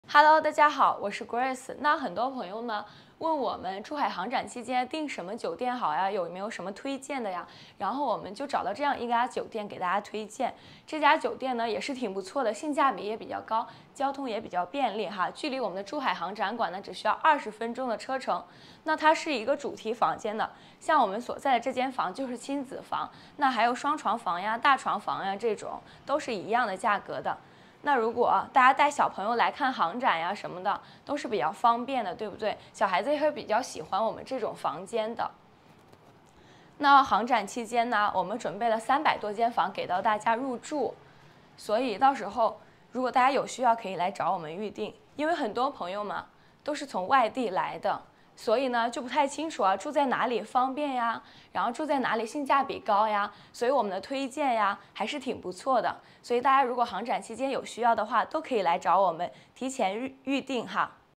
2024珠海航展优惠酒店与套票预订
这就是我们给大家安排珠海航展期间入住的酒店了，距离航展馆全程交通只需要20分钟，有很多朋友都担心我们安排的是很差的，现在大家看见了吧，这样子的主题酒店，特别那些带小孩来看航展的，小朋友肯定会喜欢上的，因为如果住在珠海航展馆对面的酒店，价格就比较跑贵，所以我们取舍后，选择性格比更高的酒店，这次珠海航展，我们每天预留300多间客服给我们的客人入住，因为大家都是来自外地，对于入住那个地方都很盲目，我们这次的安排还是很不错的，大家记得，珠海航展是很多火爆的，所以如果需要，就要抓好时间了！提前预订了